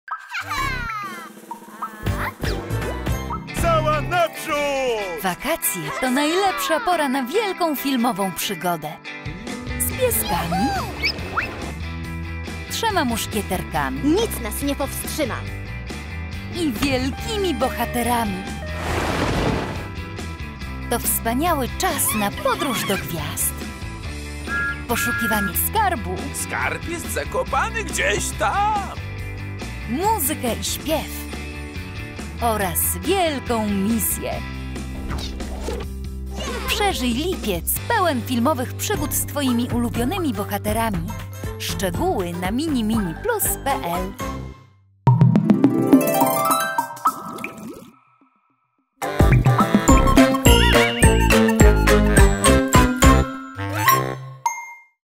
LIpiec z MiniMini+
Wakacje to najlepsza pora na wielką filmową przygodę. Zobacz co przygotowaliśmy dla najmłodszych w MiniMini+ w lipcu!

Włącz kanał MiniMini+ i oglądaj najnowsze odcinki naszych programów w tv.